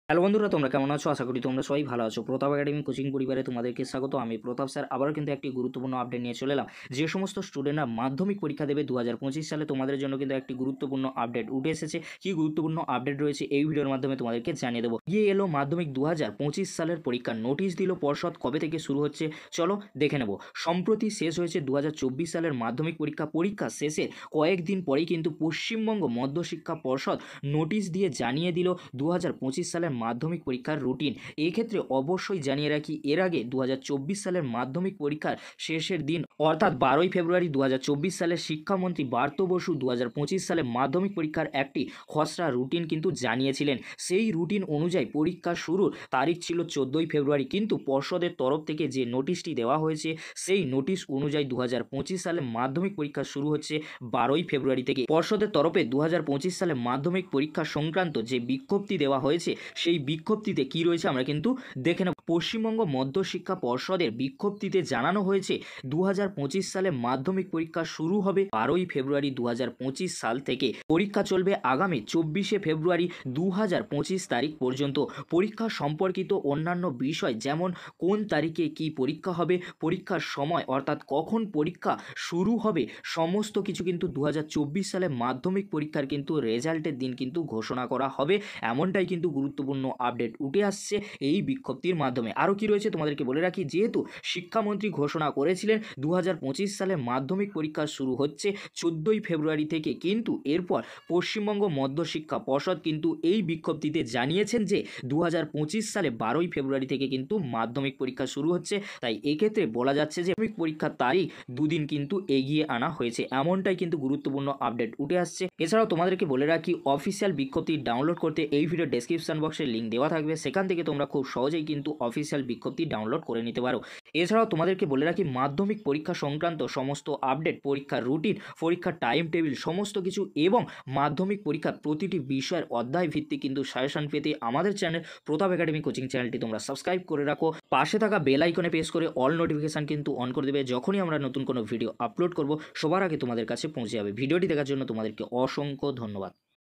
🛑মাধ্যমিক 2025 ফাইনাল পরীক্ষার রুটিন || MADHYAMIK FINAL EXAMINATION ROUTINE 2025 || WBCHSE CLASS 10
এগিয়ে এলো মাধ্যমিক ২০২৫ পরীক্ষা, নোটিশ দিল পর্ষদ! কবে থেকে শুরু হচ্ছে মাধ্যমিক পরীক্ষা দেখে নাও।।